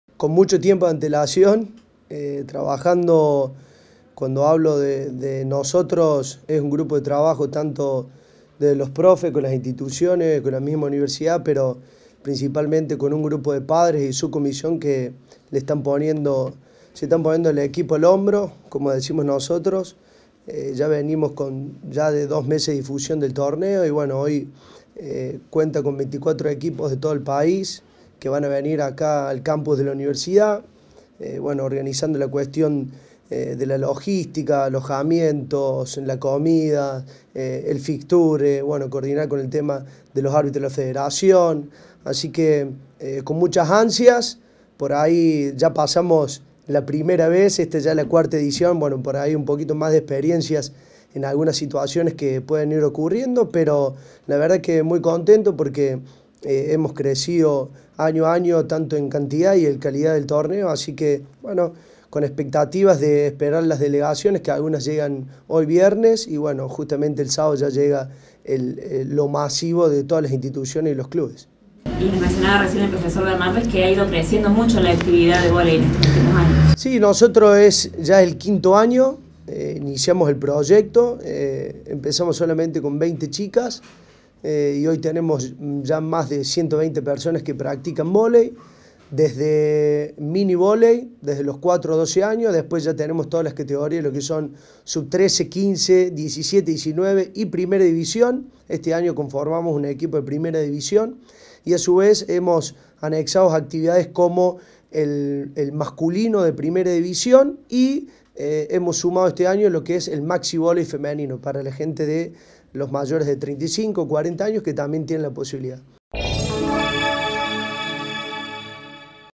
TORNEO NACIONAL VOLEY FEMENINO - MARTÍN BECERRA
ESTE FIN DE SEMANA SE REALIZARA LA CUARTA EDICIÓN EN EL CAMPUS
Facebook.com/uniriotv
Twitter.com/uniriotv